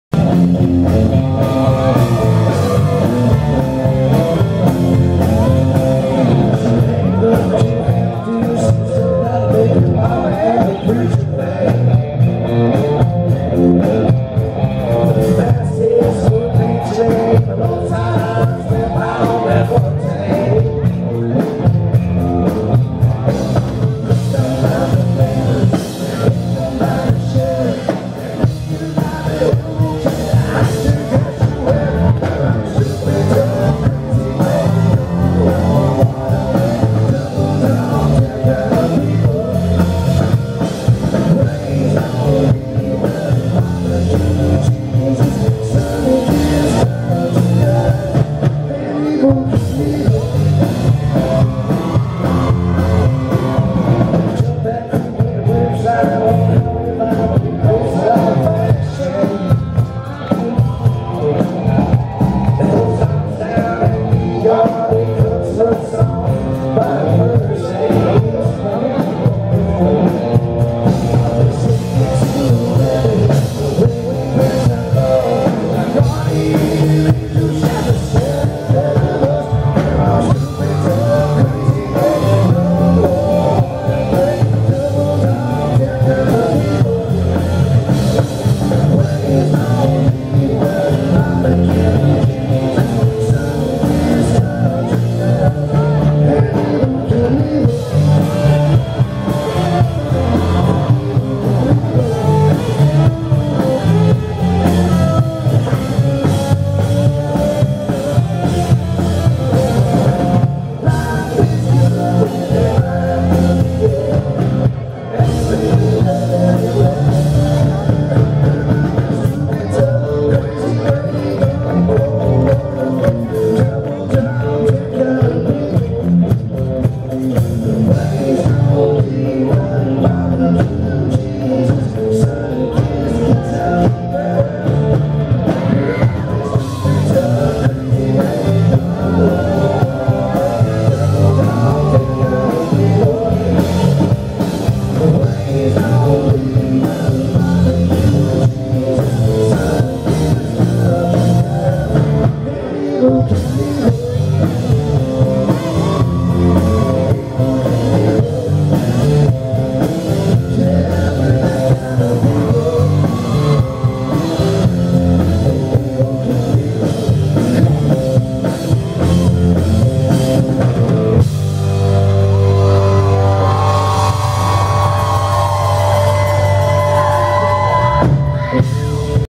Bucky Covington at the Paintsville Apple Festival 2012
Bucky performing at the Johnson Co. Apple Festival in Paintsville Ky. Oct 6th 2012